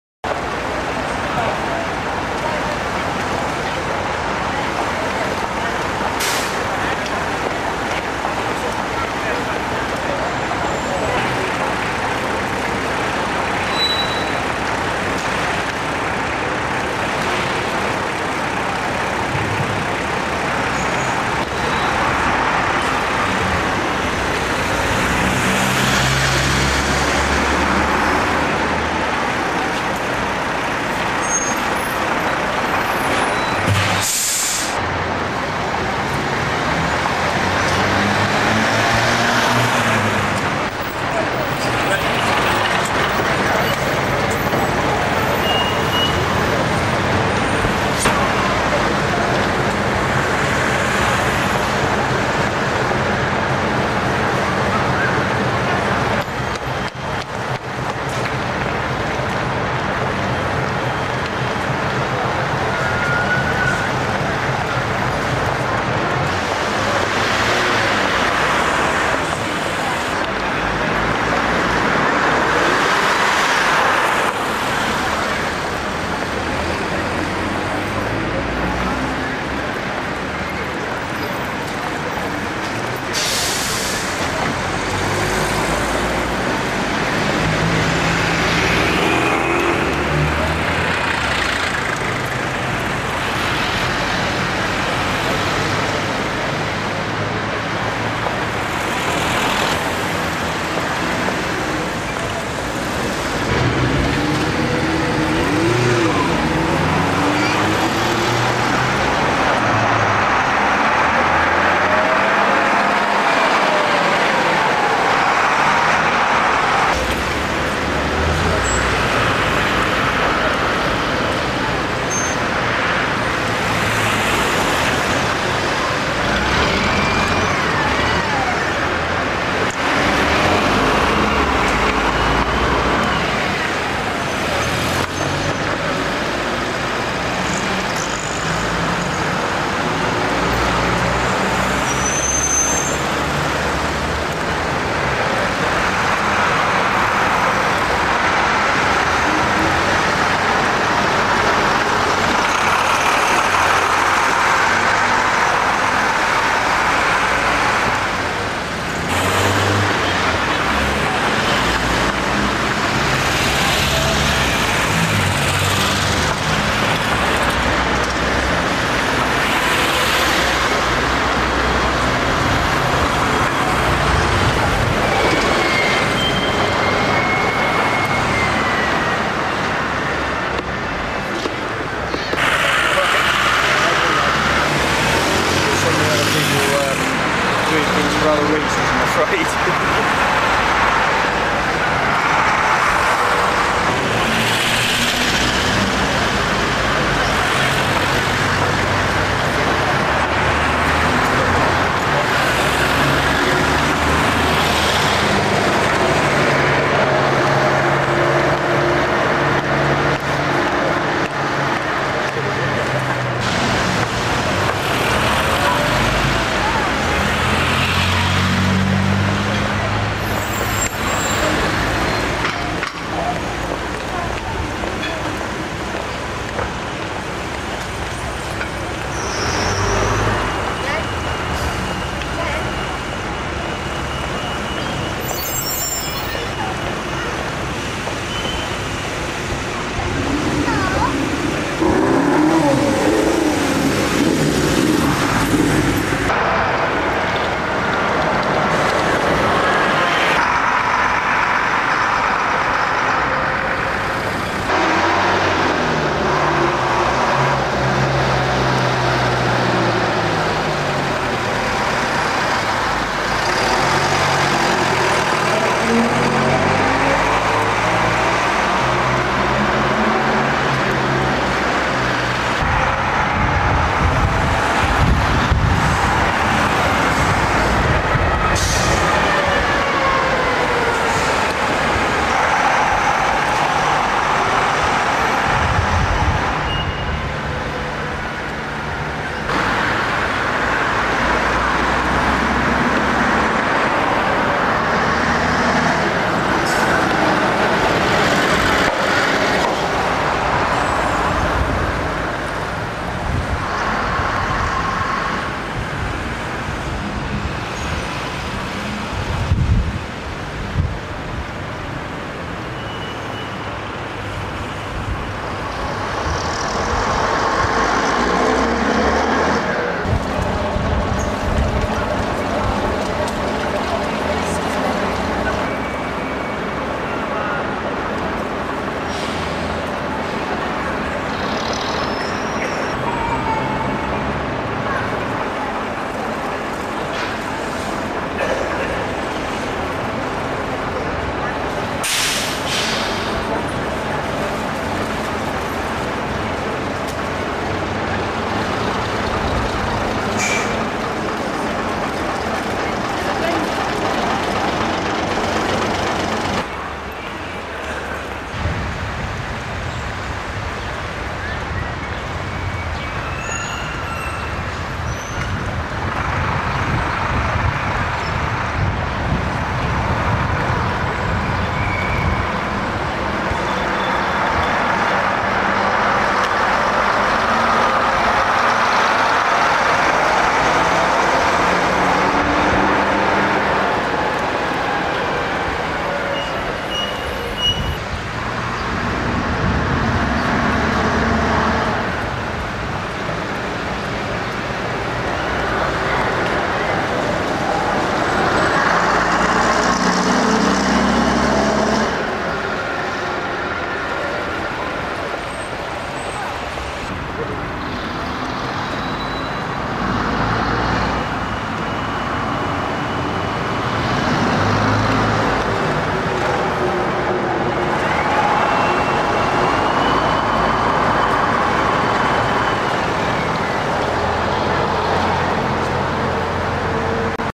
GLOUCESTER BUSES 1994
a trailer for Gloucester Buses 1990-96 , the world's largest range of transport hobby films, see website for shop, list, blog, links etc